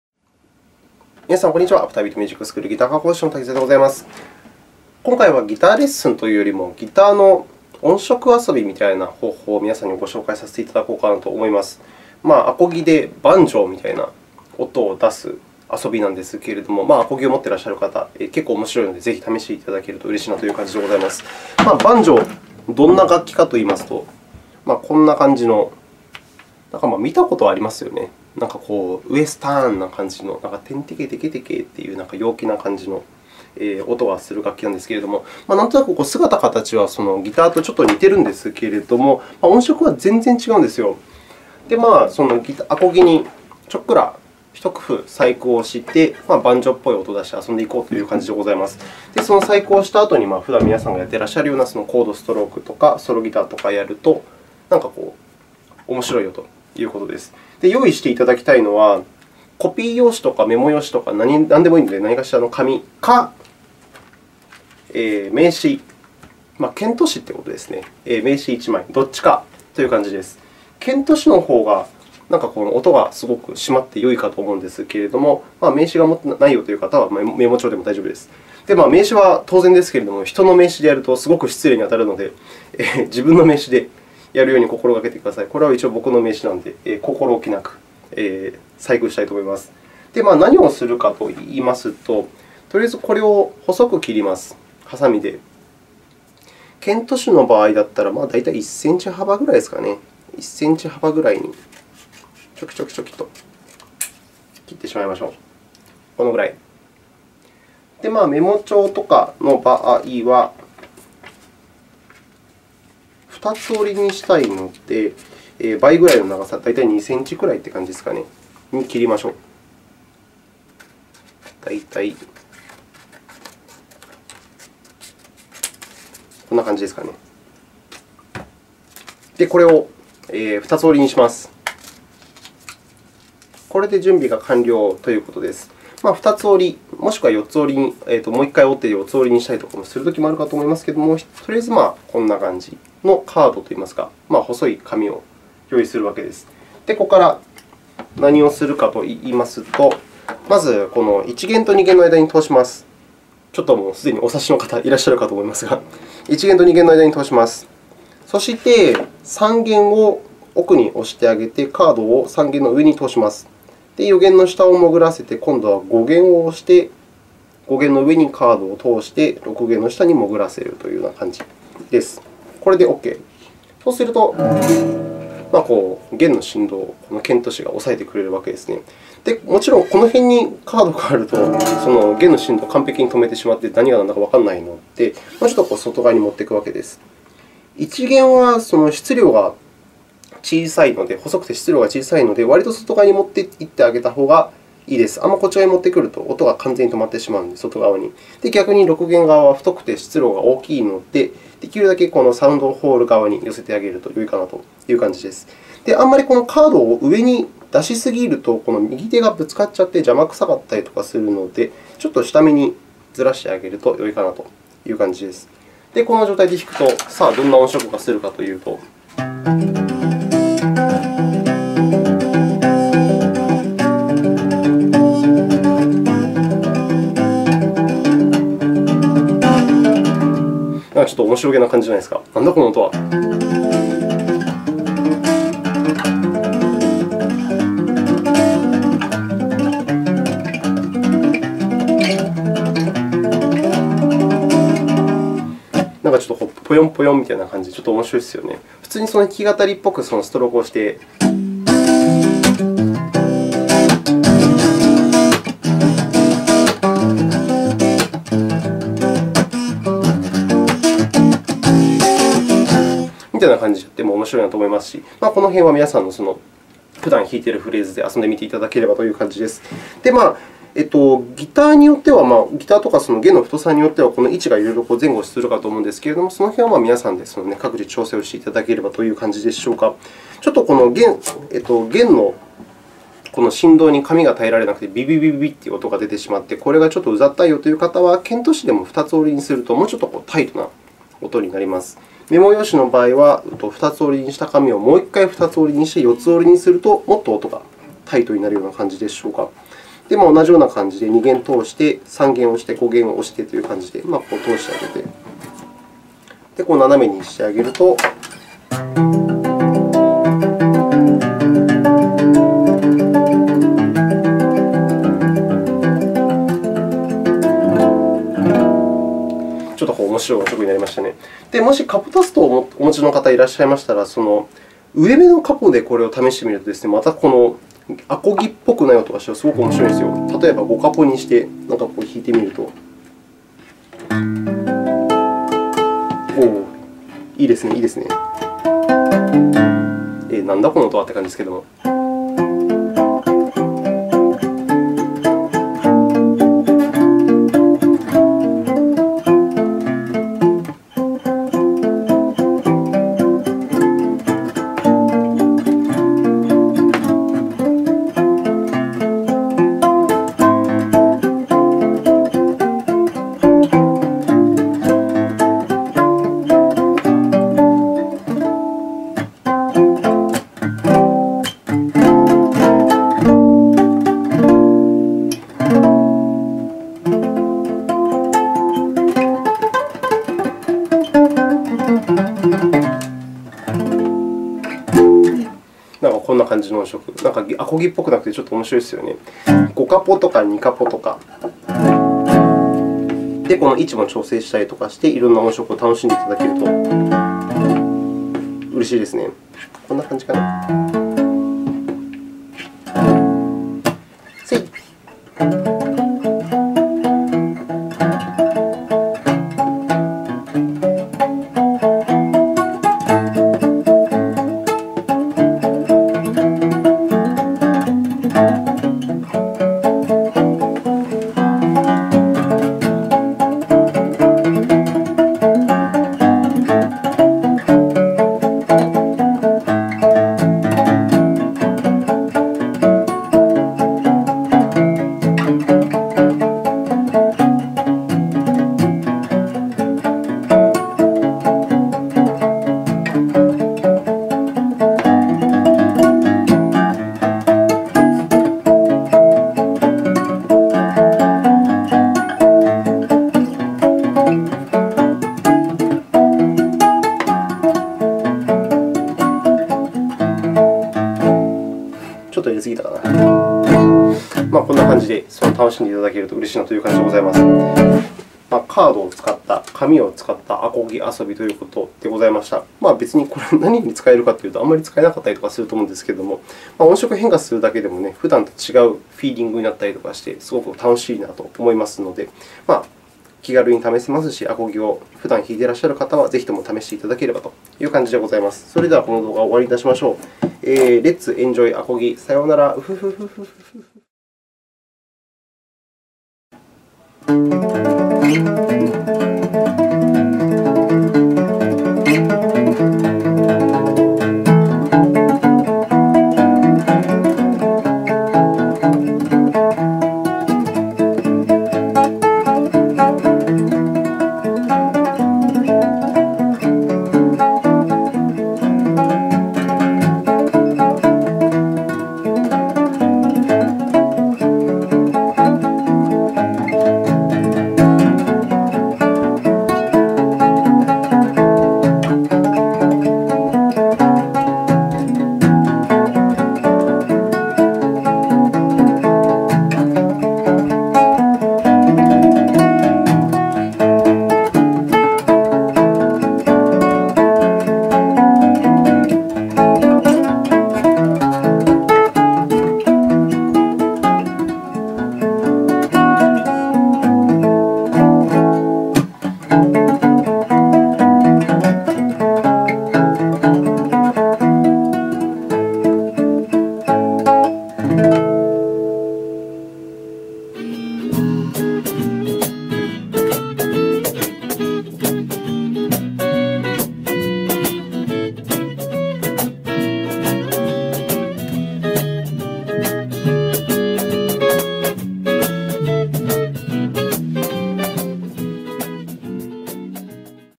ギターレッスン【簡単な細工で音色が激変！！】バンジョーごっこで遊ぼう！！
ギターにちょっと細工をするだけで音色が激変！！
手軽に細工が出来ますし、結構面白かったりするので
暇なときにでも是非試してみて頂けると嬉しいです。

もちろん細工をしたからって特別な奏法が必要になる訳では無く、
普段みなさんが弾いているようなフレーズで大丈夫です。

ぽよぽよした音色で色んな曲を弾いてみるのも楽しいですよ！！

8:14あたりからおまけで実際に演奏をしてみております。
ほぼその場でアレンジをしている様な物なので
ちょっと荒削りの演奏ですけれども、
十分参考になるかなぁなんて思ったり。

ベースとコードを同時に弾いちゃうカーターファミリー風ピッキング